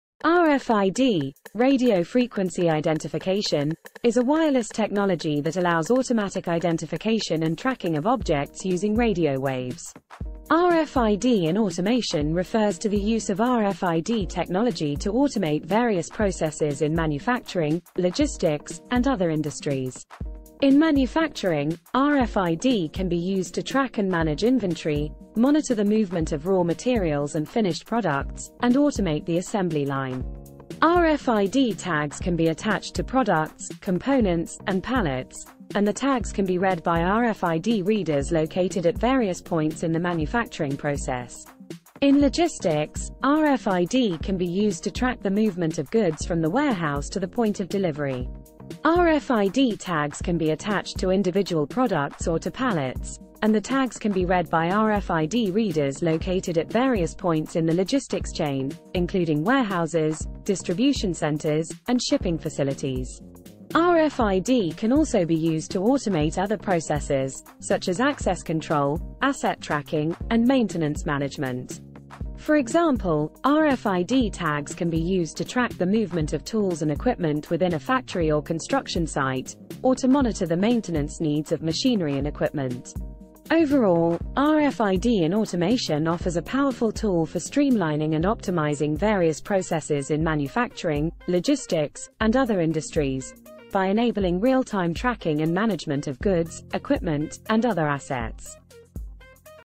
"RFID Technology Explained: How Radio Frequency Identification Works"
"Unlocking the Power of RFID: How it is Revolutionizing Industries"
In this video, we explore the fascinating world of RFID technology and how it is transforming industries across the globe. Radio Frequency Identification (RFID) is a wireless technology that enables the identification and tracking of objects using radio waves. It has numerous applications, from inventory management to contactless payments, and is becoming increasingly popular in a wide range of sectors.
Overall, this video is a comprehensive guide to understanding RFID technology and how it can benefit your business or industry. So, sit back, relax, and let's explore the world of RFID!